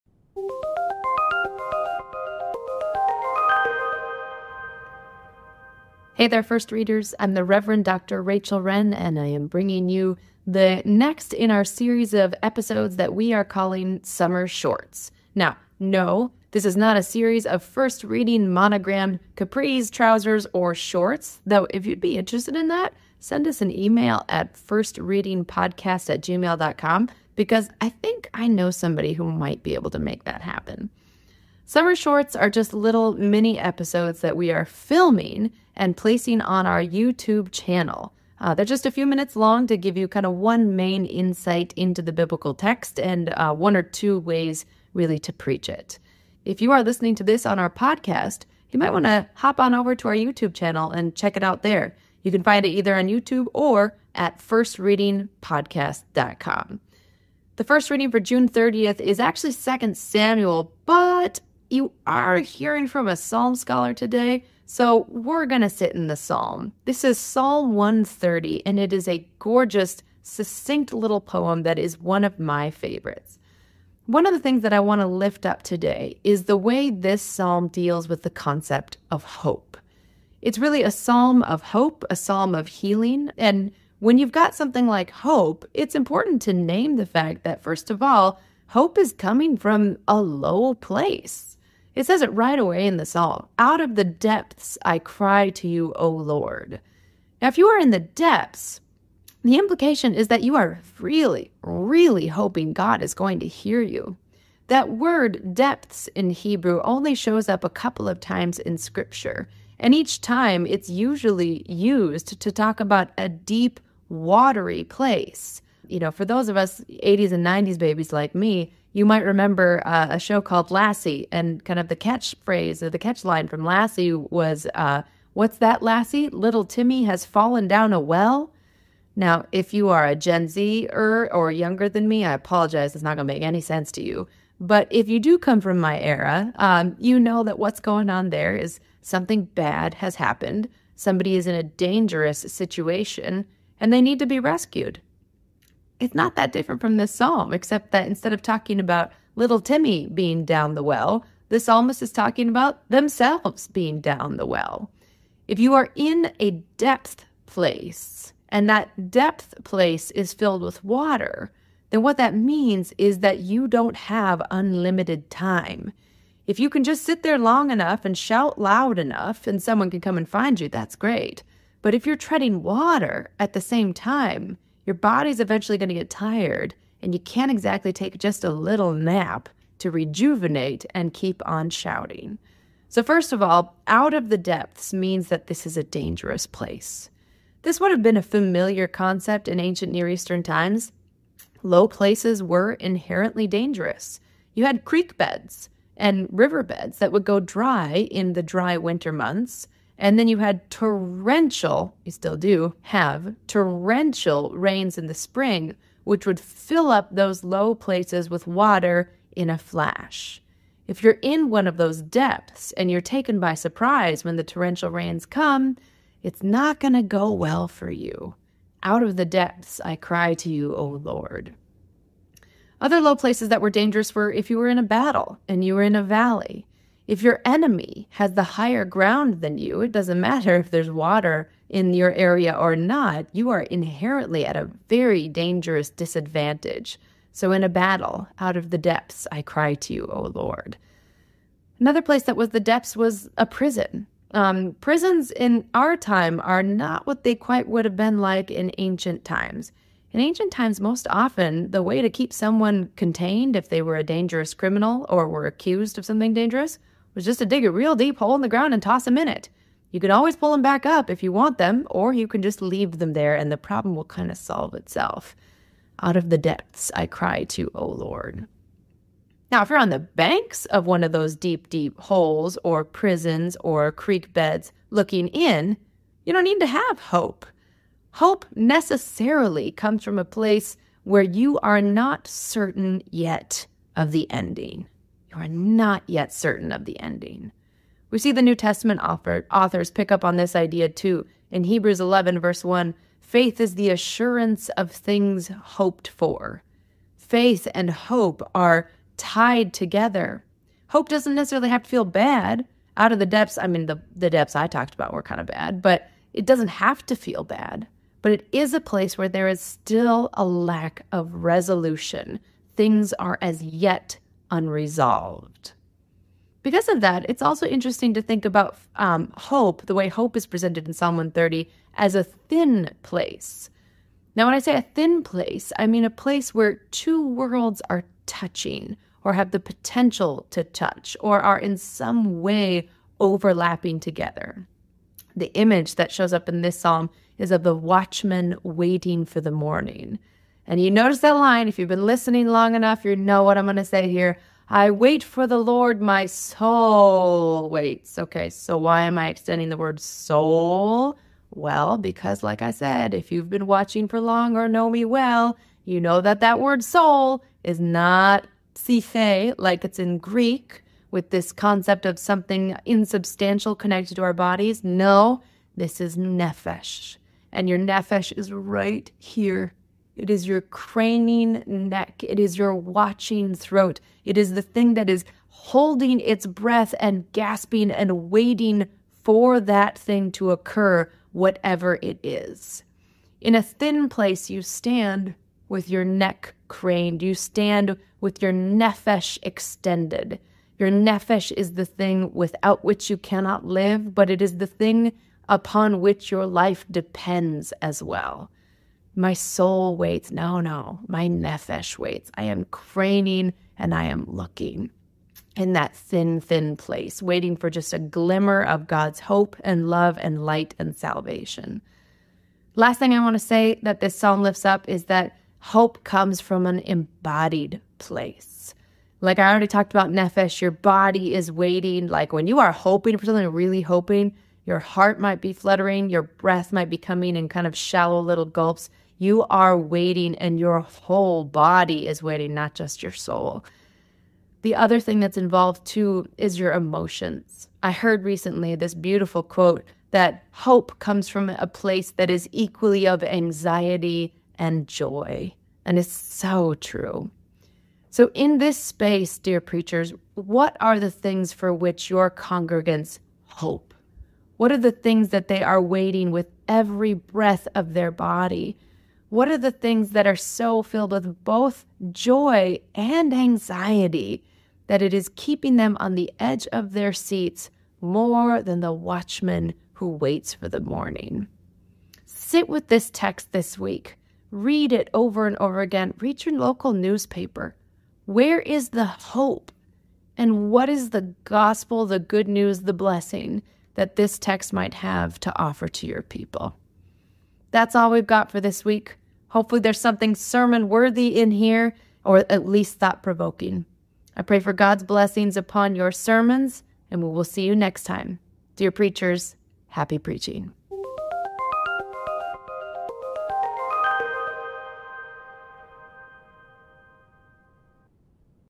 Psalm 130
First Reading "Summer Shorts"!

Lectionary Date: June 30, 2024 [6th Sunday after Pentecost, Year B]

This summer, we're trying something new: a series of video shorts with one key takeaway from the semi-continuous first reading in the RCL. This week, Rachel takes us to the depths to find some hope for our "nefesh" in the Psalm. (Did you know that we have "nefesh" t-shirts Check out the video below (and subscribe on our YouTube channel)!